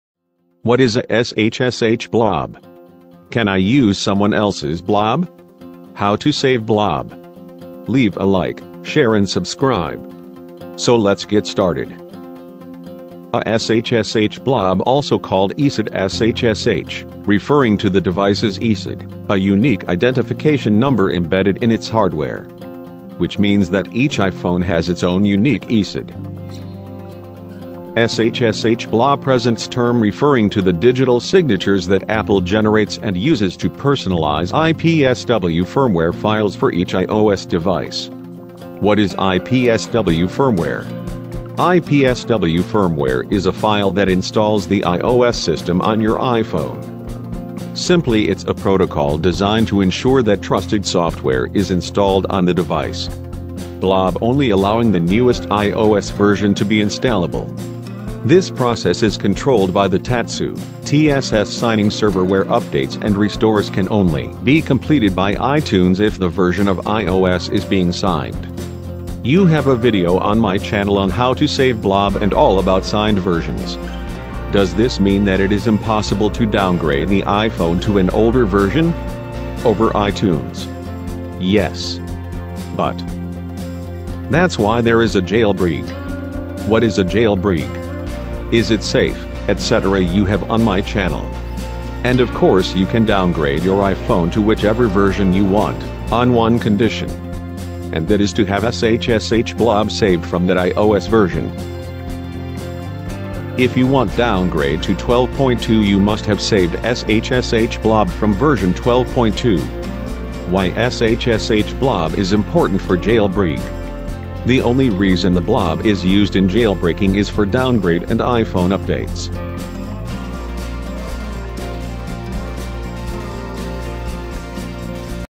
What is SHSH BLOB? All about blobs?
0:00 - Intro
0:12 - What is SHSH BLOB?
0:38 - What is IPSW FIRMWARE?
0:47 - BLOB in Installation process
0:58 - TSS Server
1:09 - How to save BLOB?
1:14 - Can downgrade Iphone to older ios version?
1:26 - What is a JAILBREAK?
1:32 - Can downgrade Iphone to older ios version?
1:50 - Why SHSH BLOB is important for JAILBREAK?
2:00 - Outro

PIECE